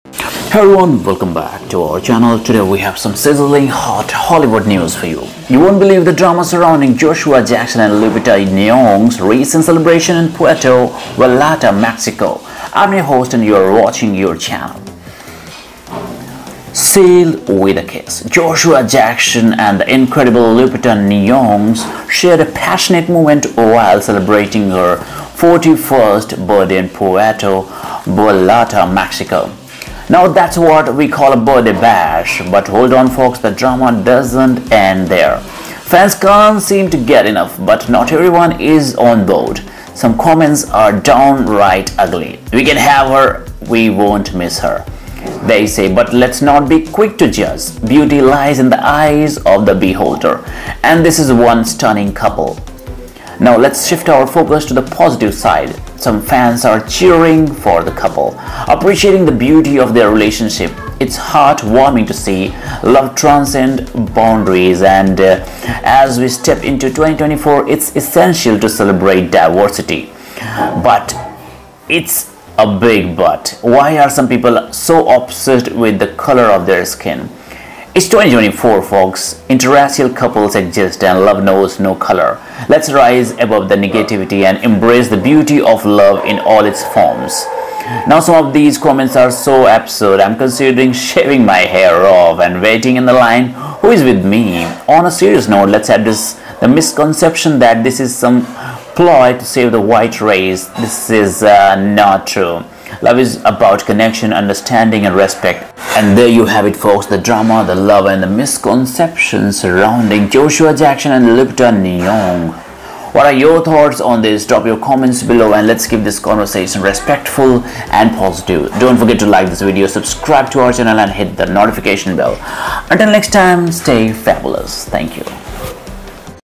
Joshua Jackson And Lupita Nyong'o Passionately Kissed Celebrating Her 41st Birthday In Mexico
celebrityfactsnsecrets Sealed with a kiss. JoshuaJackson and girlfriend Lupita Nyong'o passionately kissed while celebrating her 41st birthday in Puerto Vallarta, Mexico. Get the details